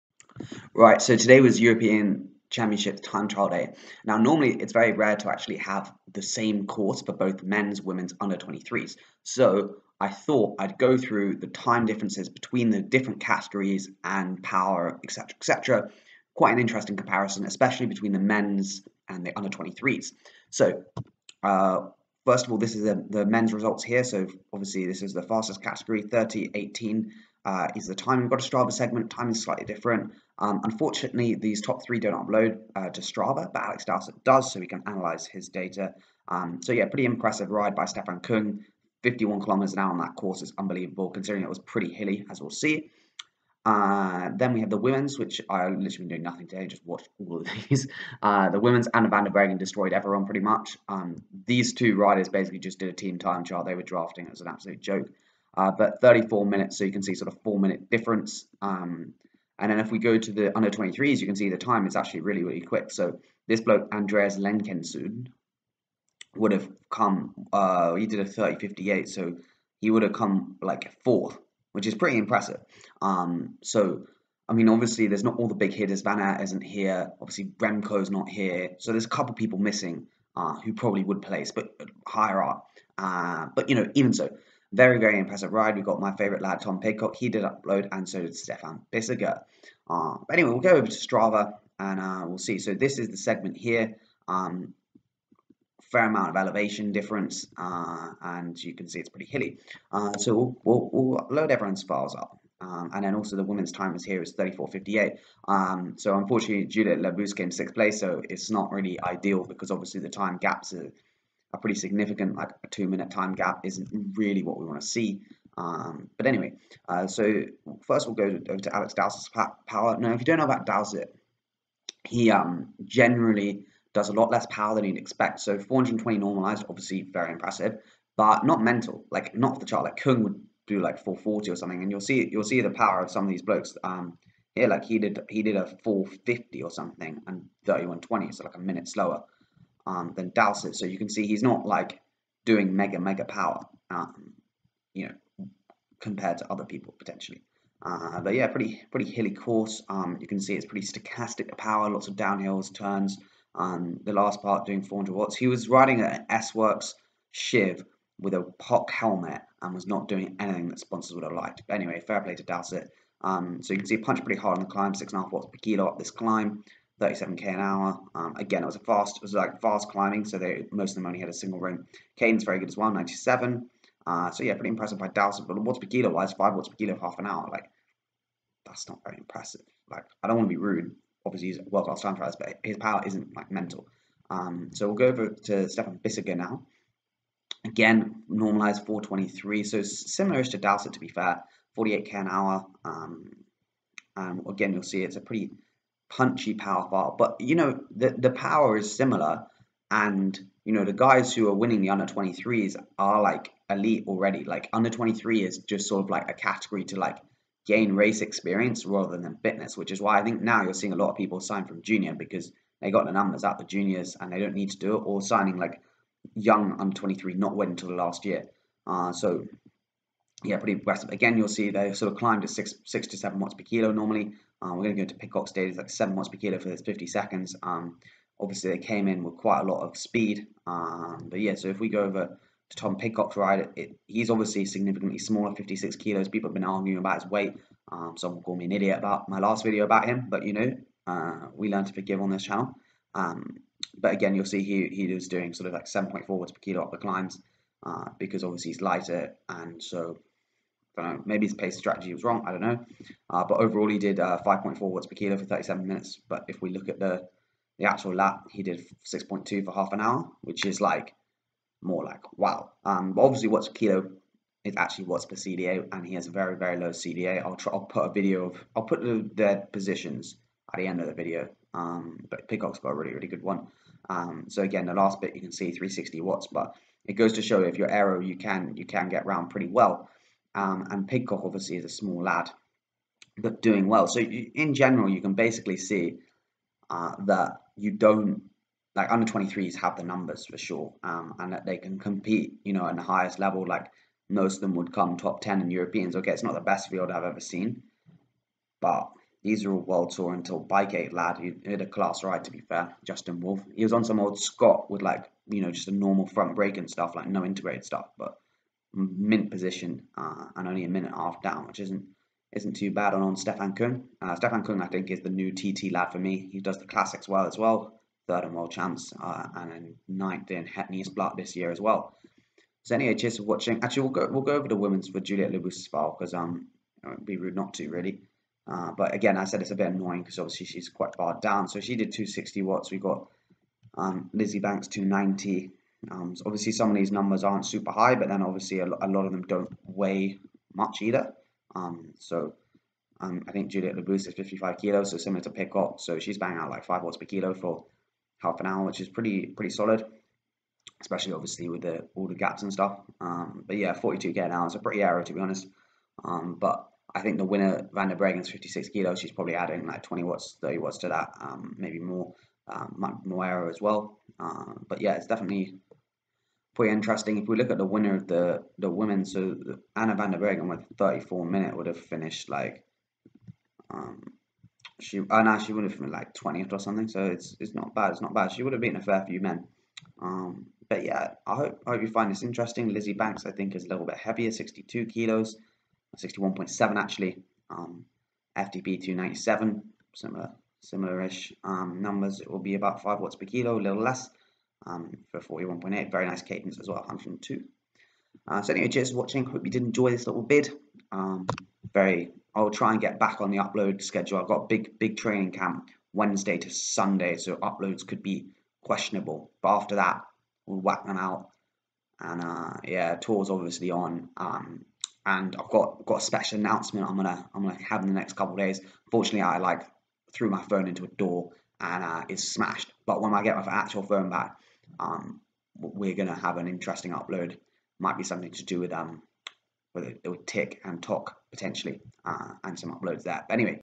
European Time Trial Champs POWER COMPARISON
How much faster are the pros vs the u23s and how much faster are men vs women. This is all answered by analysing the 2020 European Time Trial Championships that took place today